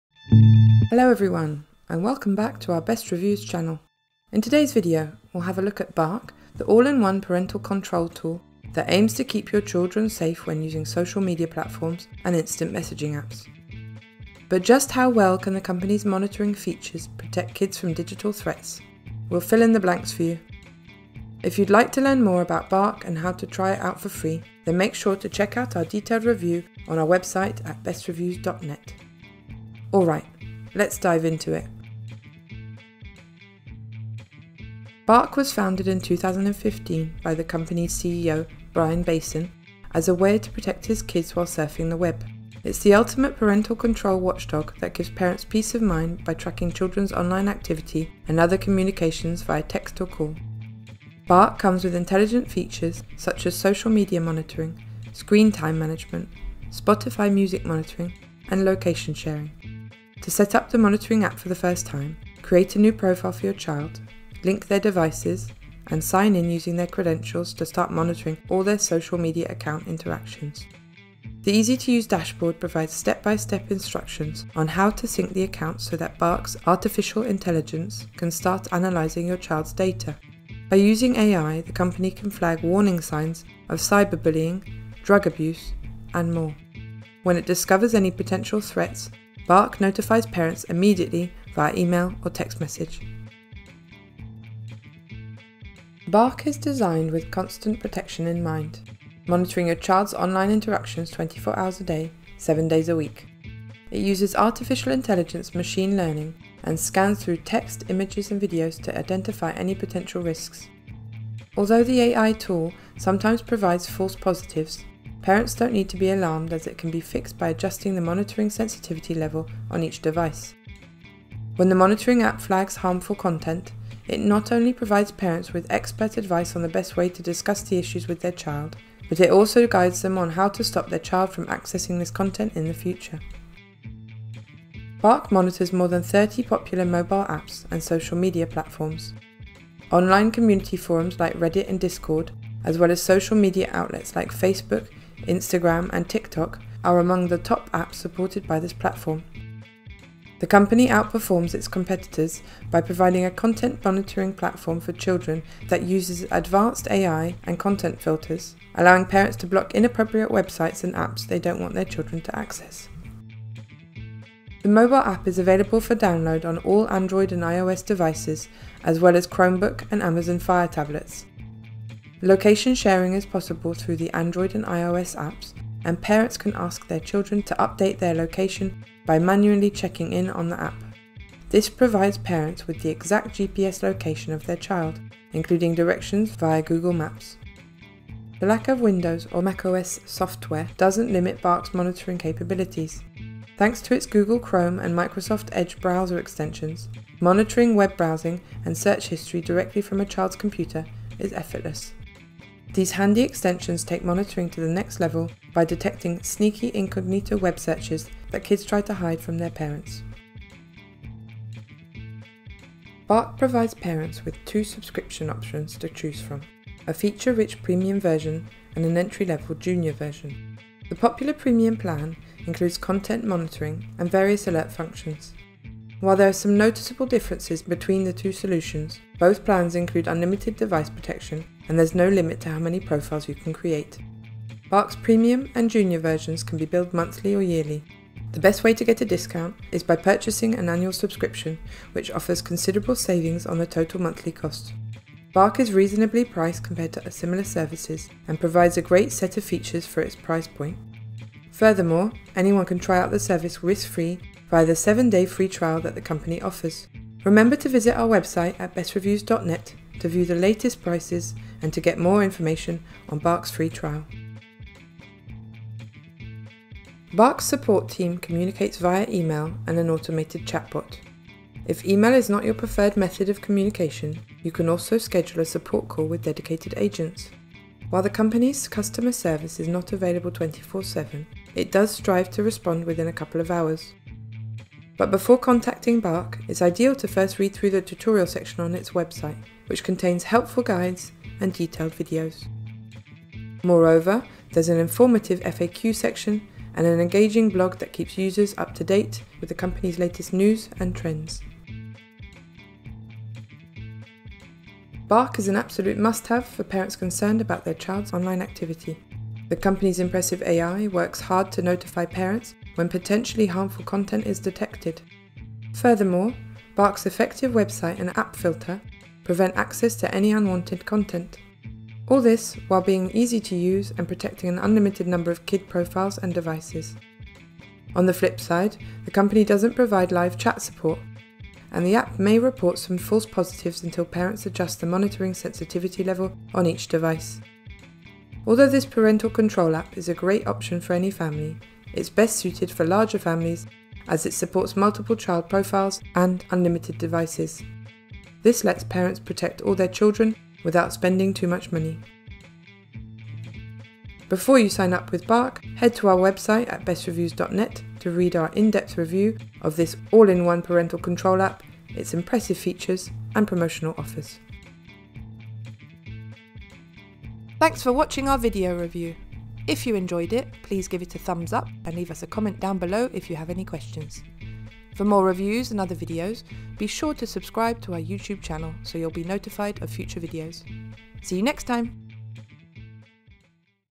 Review | Best Parental Control Software Reviews
Are you worried about the digitalsafety of your child and want to shelter them against harmful online content? If this is the case, then the all-in-one parentalcontrolsoftware, @BarkTechnologies might be the right solution for you. It has impressive monitoring features and supports over 30 apps and social media platforms, meaning you can rest assured that your kids are browsing and communicating with others safely. If you’re ready to learn more about this parental control tool, check out our video.

⏱️ Time Stamps

0:00 - Introduction
0:35 - Walkthrough
1:36 - Features
3:33 - Pricing, Trials & Deals
4:27 - Customer Support
5:05 - Conclusion